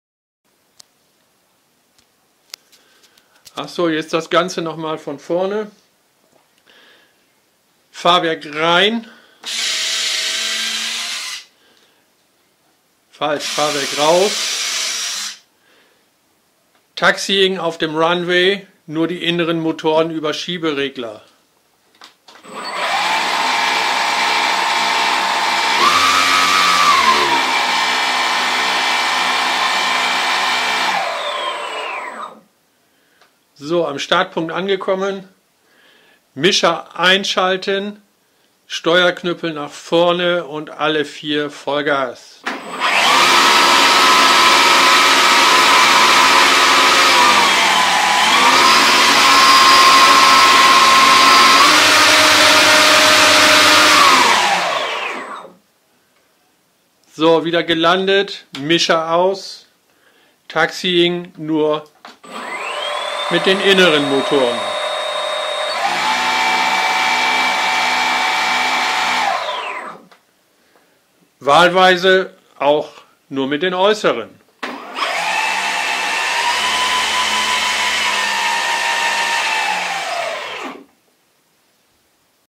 Rc-Warbird-Produktion B-17 Motorsteuerung
Ich habe mal die 4 Motoren am Sender auf 2 Kanäle (3 & 8) gelegt, einen freien Mischer erstellt und zusätzlich einen Schieberegler (K. 8) für die 2 inneren Motoren geproggt.
Sinn war bzw ist es, auf dem Feld zum taxiing nur mit den 2 inneren Motoren zu fahren; im Flug sind dann alle 4 Motoren per Stick normal zu steuern.

english:
i did the 4 motors on 2 channels ( 3 & 8 ) and progged a mixer for them with a logicswitch.Second i put the channel 8 on a slider. Now i'm able to taxiing per slider only with the inner motors; to start the b-17 i've to switch on the mixer and have all 4 motors on my gasstick.